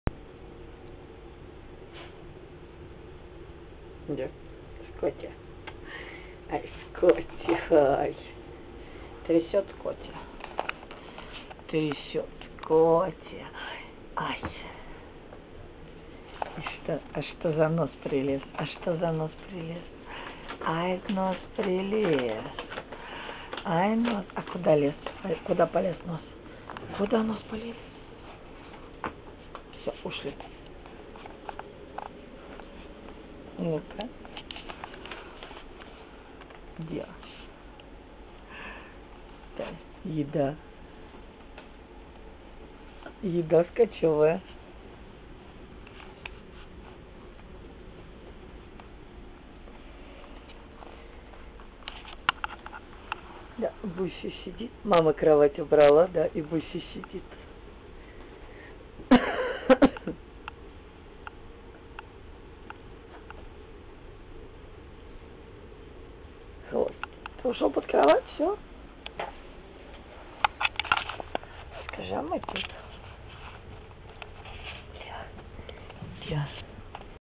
Travelling Scotties
In the cabin on board Viking Line, Helsinki - Stockholm